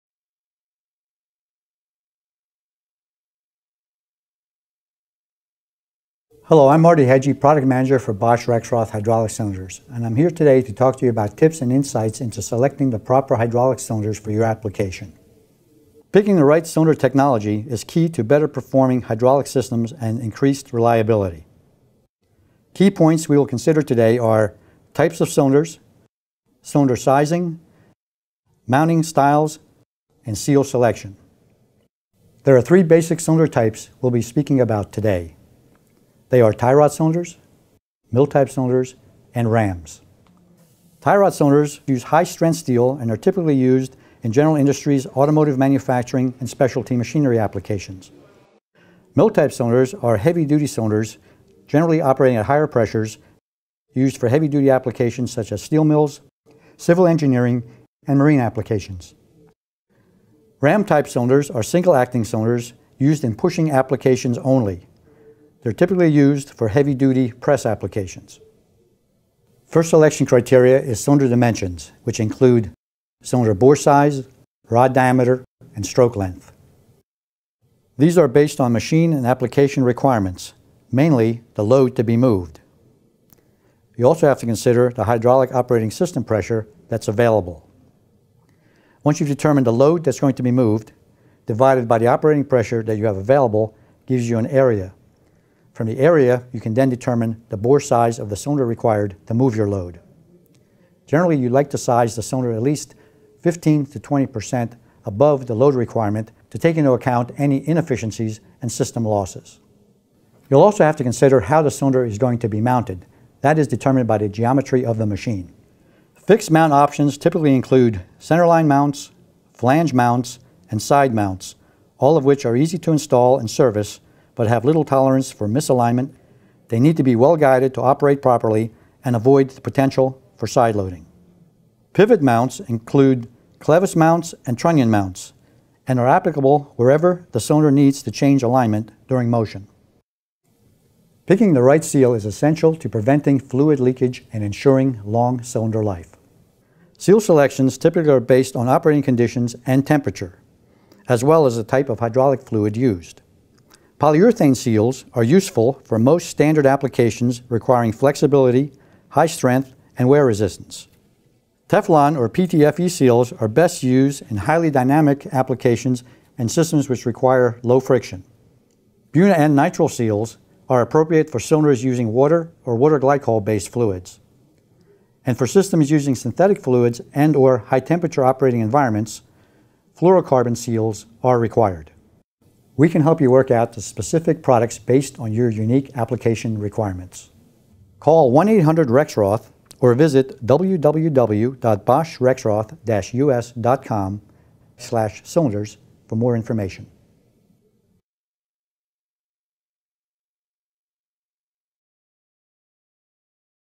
Cylinder Selection: The Key to Better Hydraulic Systems
Bosch Rexroth provides tips and insights into selecting hydraulic cylinders. In this how-to video you will learn about the different types of cylinders (tie rod, mill type and ram), sizes, mounting styles and seal selection.